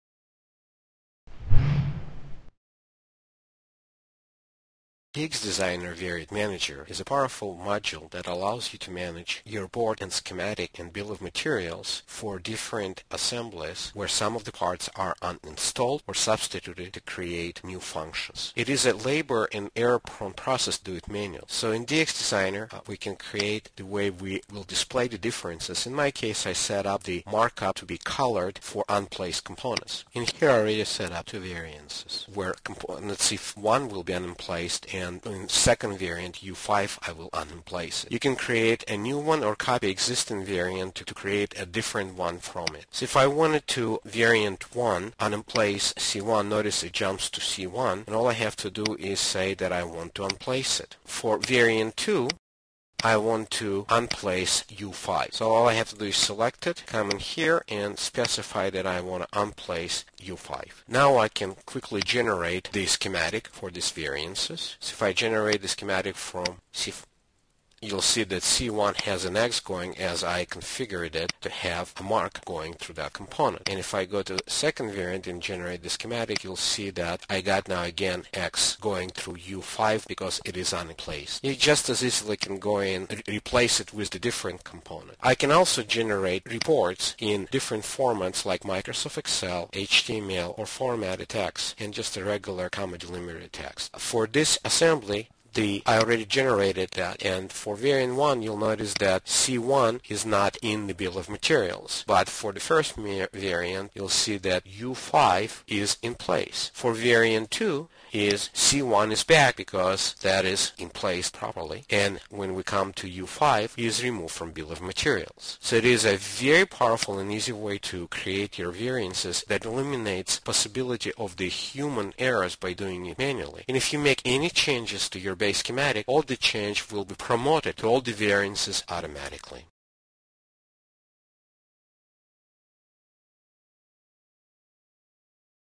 Variant Manager for the PADS Flow
Variant Manager helps to automatically manage different build options for Assembly, BOM and Schematics, helping you avoid wasted time and costly mistakes involved with doing this task manually. Quickly create from the base schematic master variants based on install/uninstall, or easily substitute any approved component on a given board.